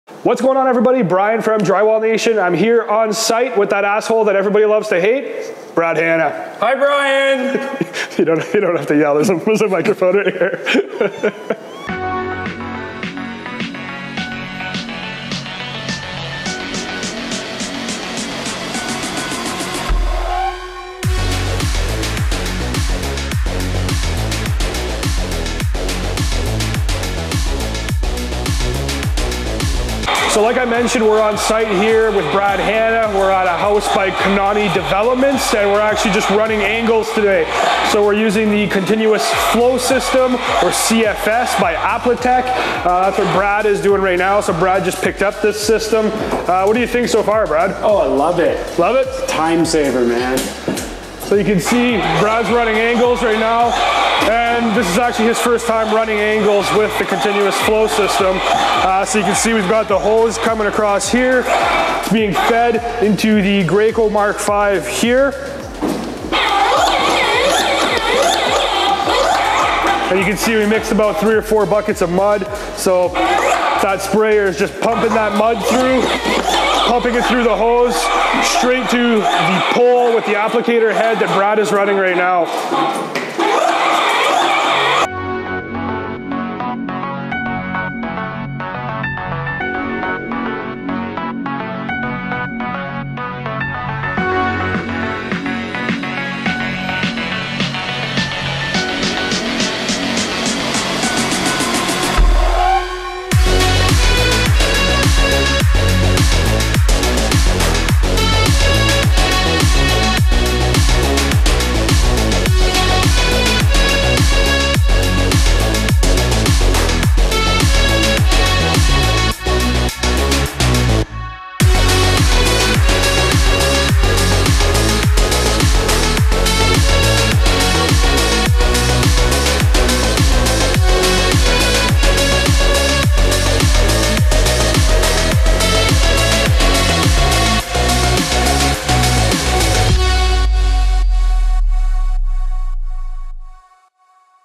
Apla-Tech CFS Continuous Flow System w/ Brad Hanna
We stopped by to visit our friend Brad Hanna on one of his jobsites.
We coated angles with the Apla-Tech CFS Continuous Flow System using the Graco Mark V.

All products seen in this video are available at CSR Building Supplies